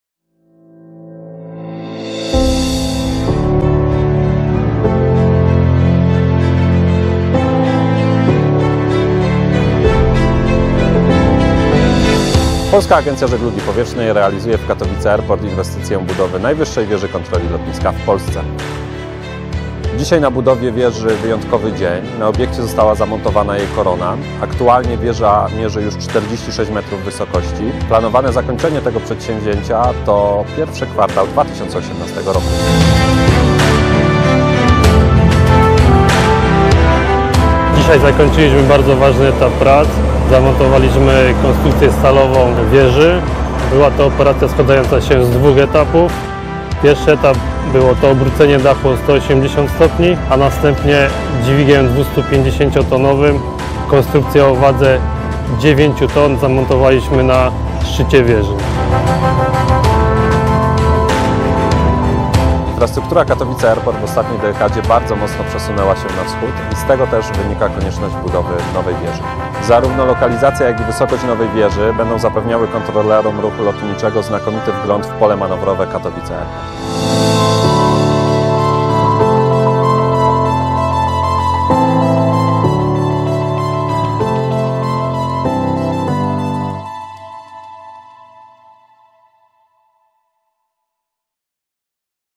"Koronacja" najwyższej wieży kontroli lotniska w Polsce
W sobotę 14 października 2017 roku w KTW doszło do "koronacji" najwyższej wieży kontroli lotniska w Polsce. Inwestycję na zlecenie Polskiej Agencji Żeglugi Powietrznej realizuje Budimex SA